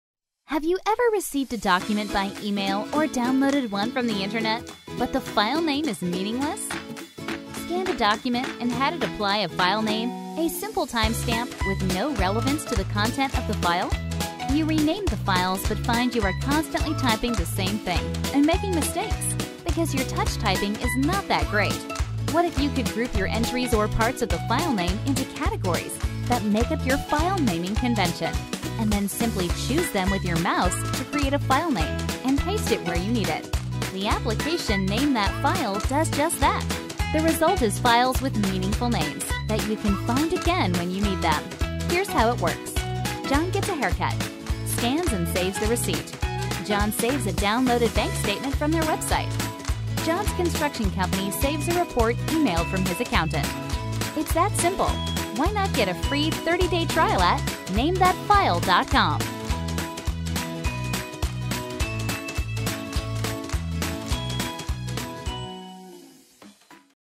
File naming with NameThatFile
Need an easy way to consistently name your files.
Don't rely on your touch-typing, but select from customised lists to create your file name.